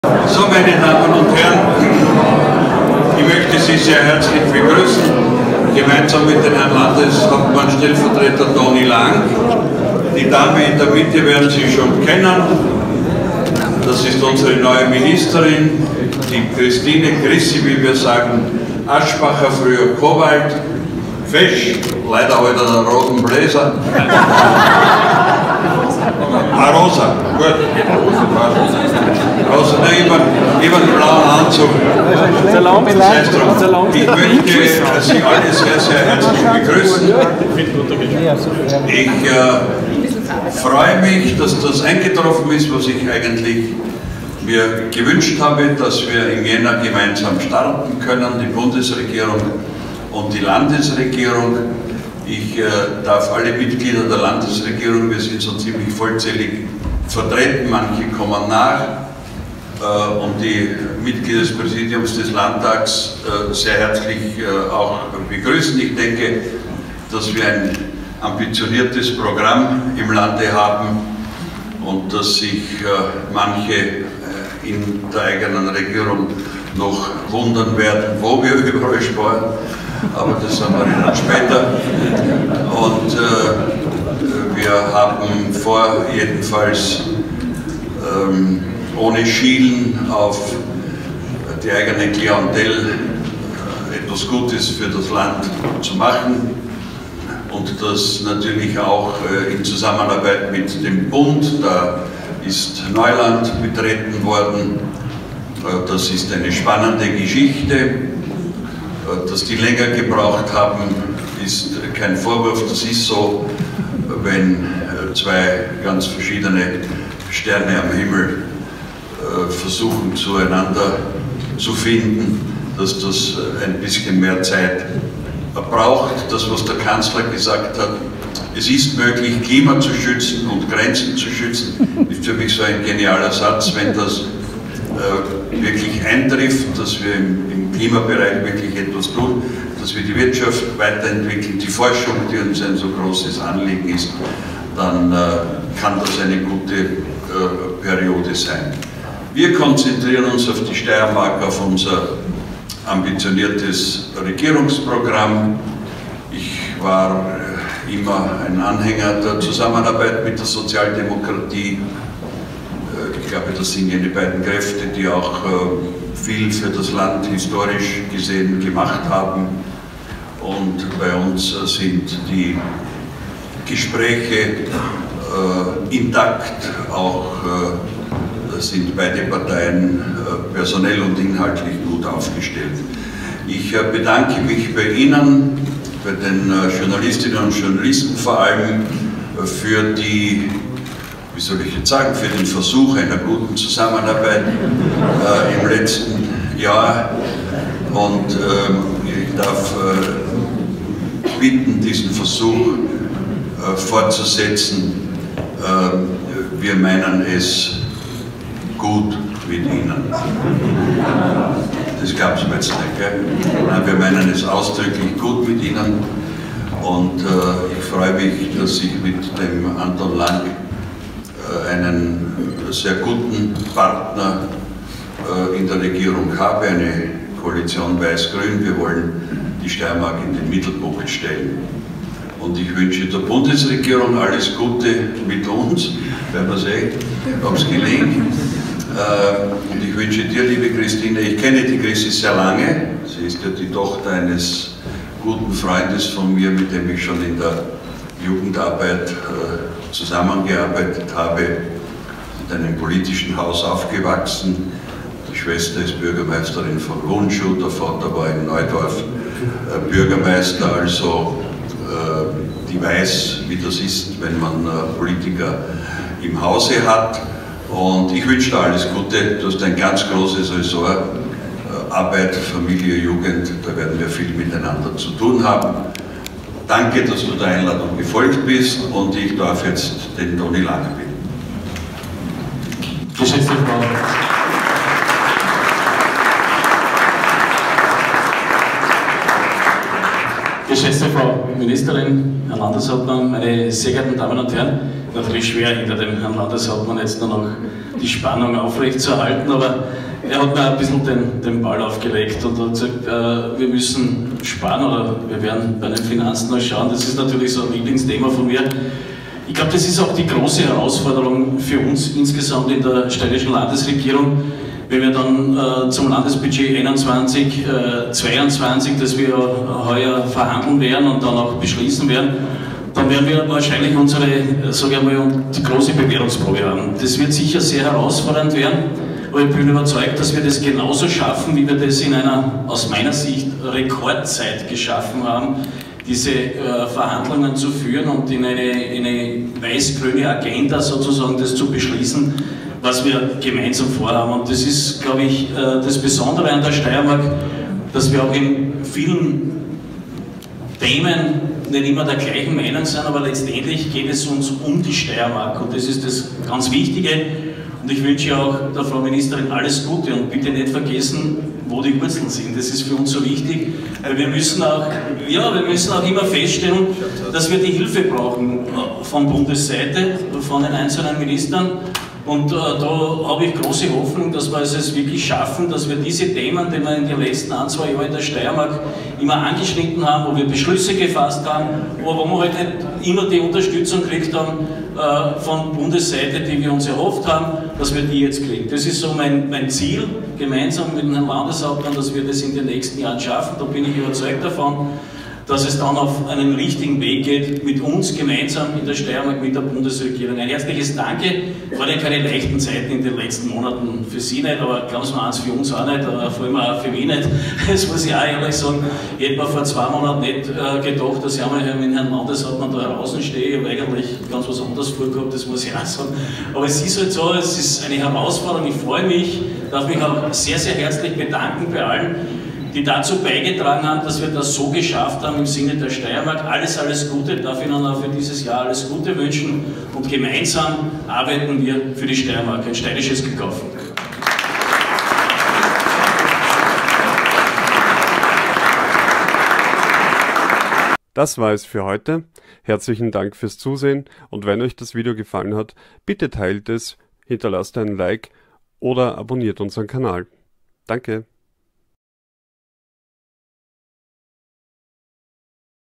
Medienempfang des Landes Steiermark - Landeshauptmann Hermann Schützenhöfer - On The Grid Ep: 241
Die steirische Landesregierung lud am 9. Jänner zum traditionellen zum Medienempfang zum Jahresanfang ein. Dabei sind Medienvertreter und Herausgeber, die Landesregierung als auch Presseverantwortliche des Landes, der Behörden, Sozialpartner und der Wirtschaft eingeladen.

Landeshauptmann Hermann Schützenhöfer und sein Stellvertreter Anton Lang begrüßten dabei auch die neue Familien- und Arbeitsministerin Christine Aschbacher. Während der Rede betonte Schützenhöfer gegenüber den Medien dass es die Landesregierung "ausdrücklich gut" mit der Presse meine, ein Kommentar der für Gelächter unter dem Publikum sorgte.